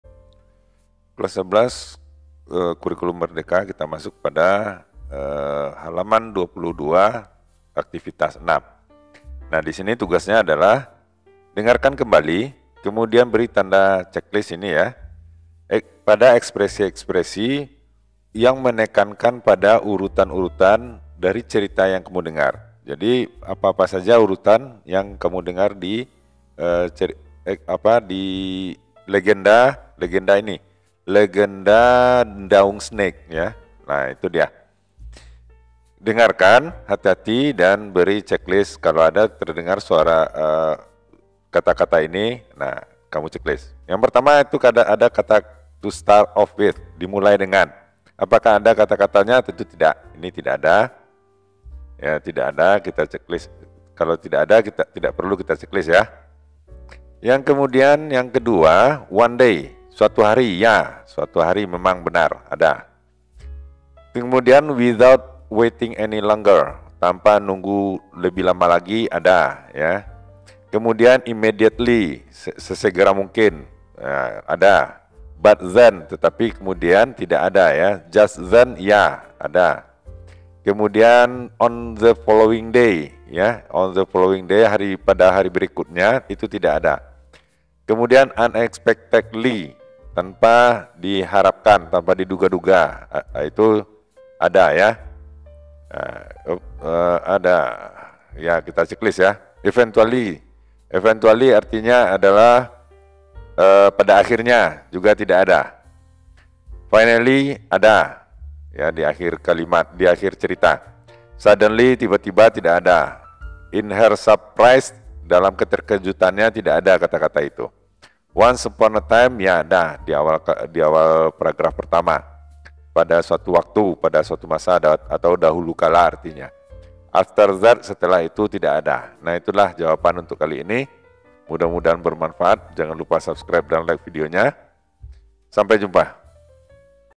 Hal. 22 kelas 11 Terjemahan Buku Bahasa Inggris SMA SMK Kurikulum Merdeka
Activity 6
Ceklis kata atau ekspresi yang menunjukkan urutan waktu pada Legenda Ular Ndaung.

terjemahin yuk
terjemahinyuk
terjemahkan yuk
Kurikulum Merdeka
Kelas XI
Tingkat Lanjut halaman .